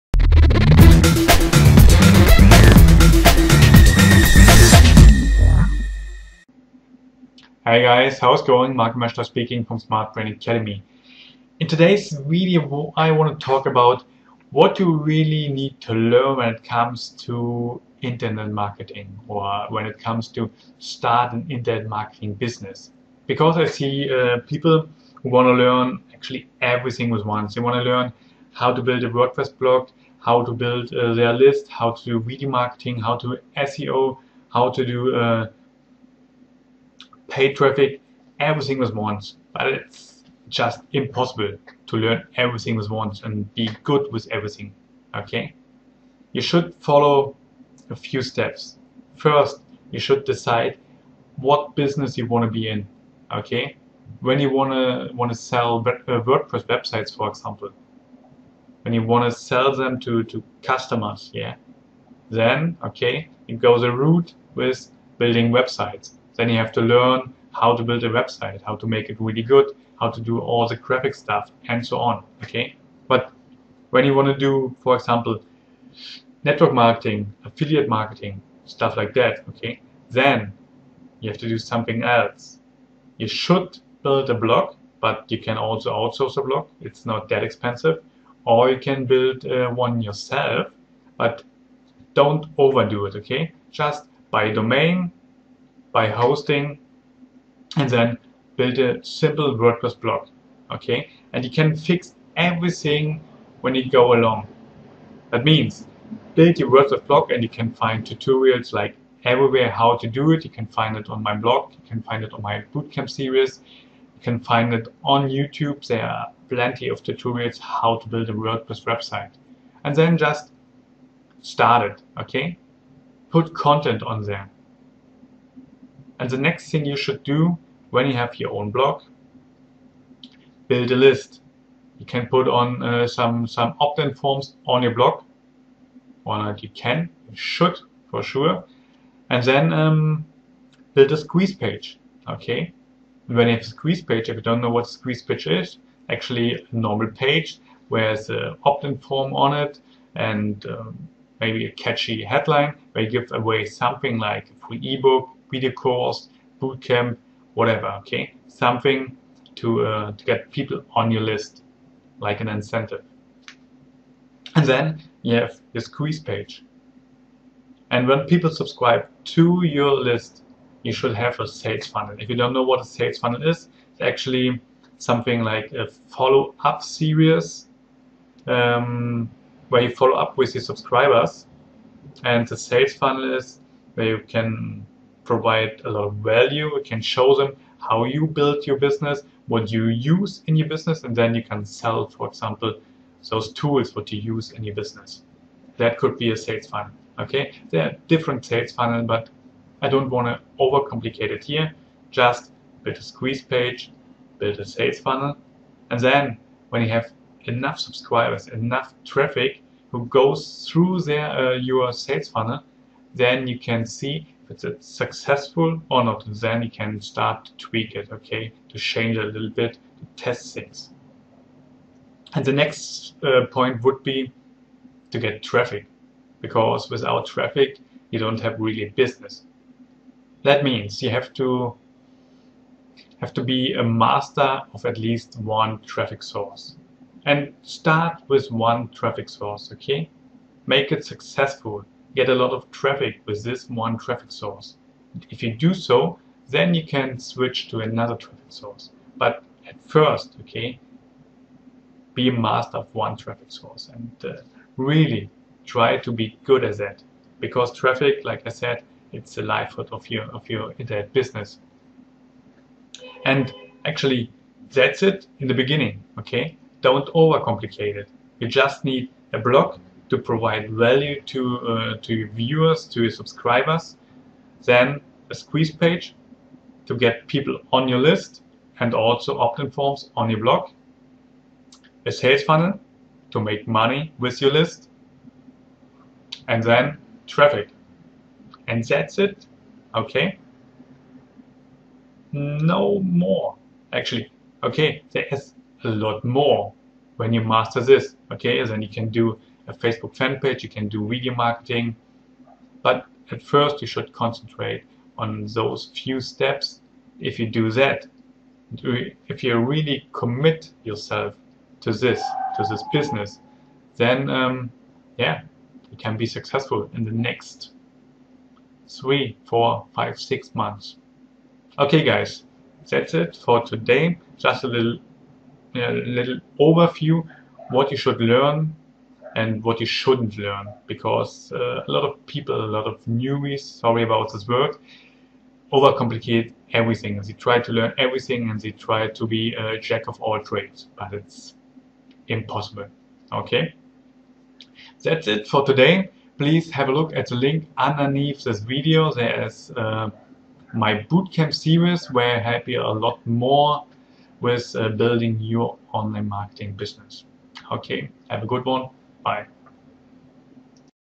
What should you first learn when you start your own Internet Marketing Business
What should you learn when you start your own internet marketing business? And the answer would surprise most people because they think they have to learn tons of stuff and everything with once. But when you start a online business you should just concentrate on a few things to get started and all the rest can wait for later. Watch the video where I explain what you need to learn first to start your own business.